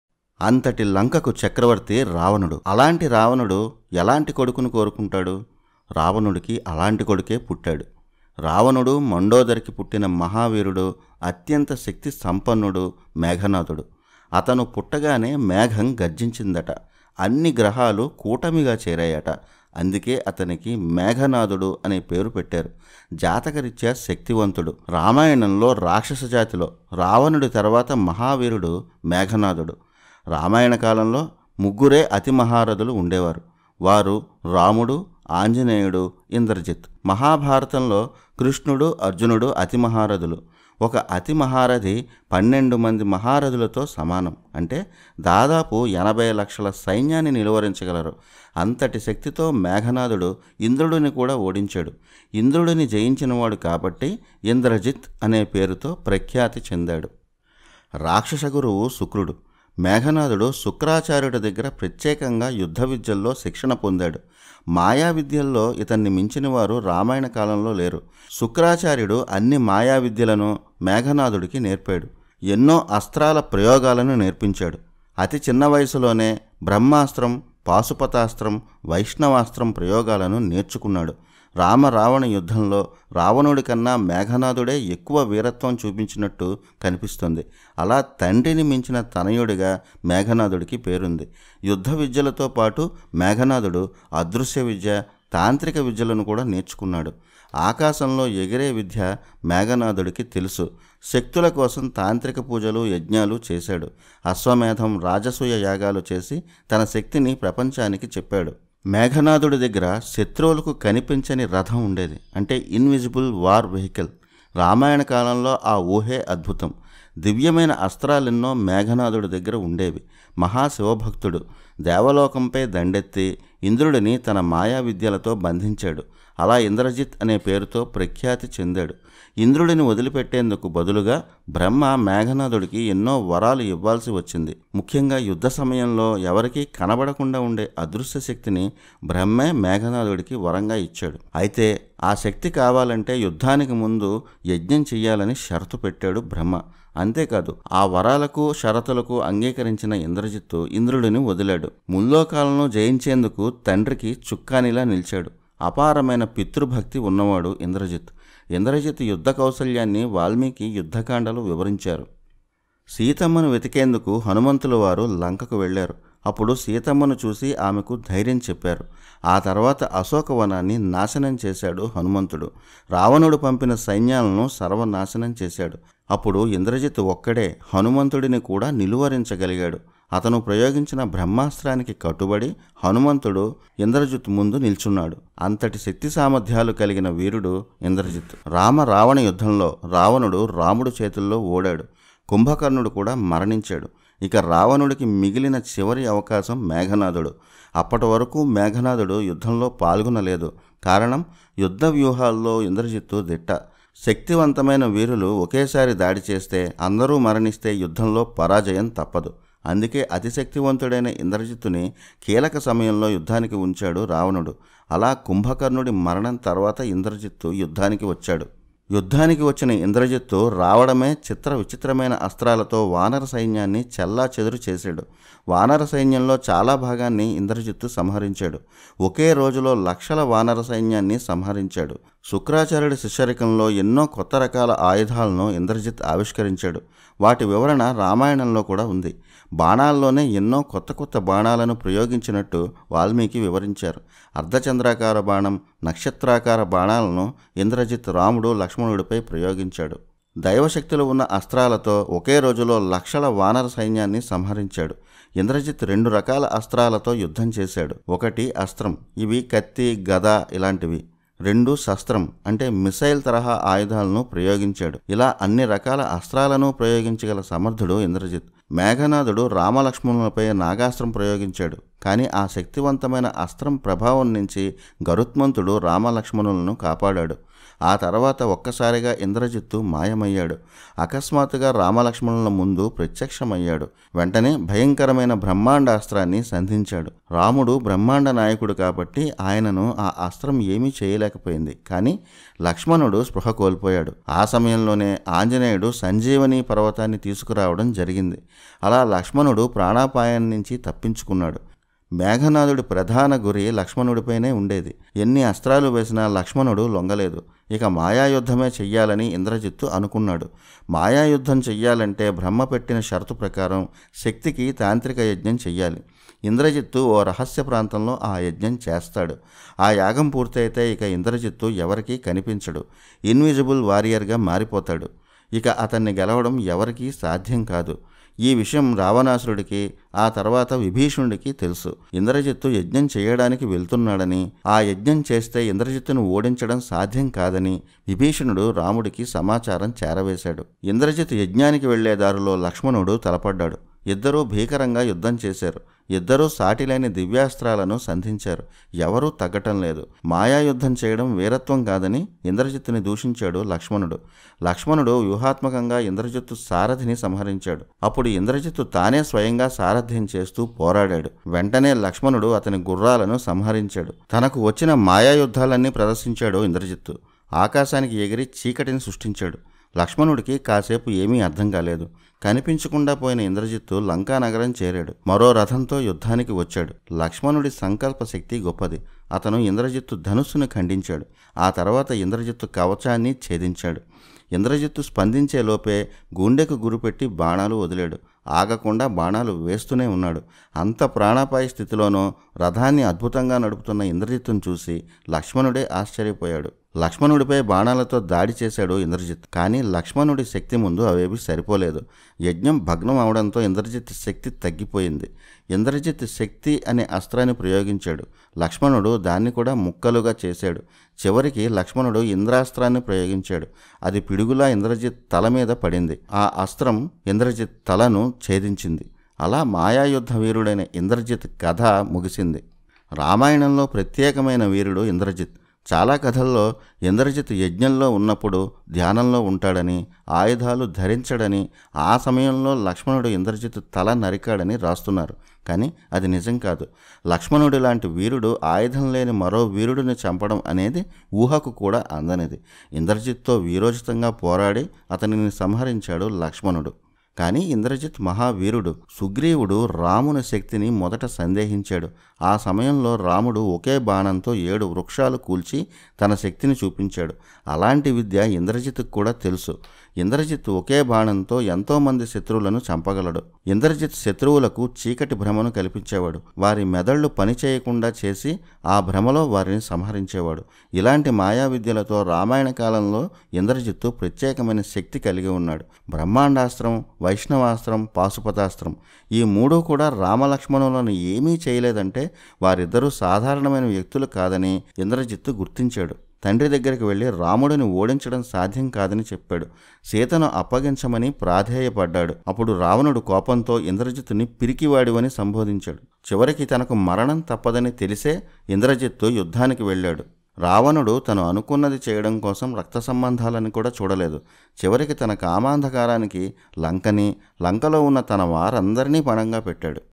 ఇంద్రుడినే ఓడించిన వీరుడు, రావణాసురుడి కుమారుడు ఇంద్రజిత్ | Ravana Son Indrajit Story in Ramayanam
ఇంద్రుడినే ఓడించిన మహావీరుడు రావణాసురుడి కుమారుడు ఇంద్రజిత్ గురించి తెలియని నిజాలు..

భక్తి పాటలకోసం కింద ఉన్న లింక్ క్లిక్ చేయండి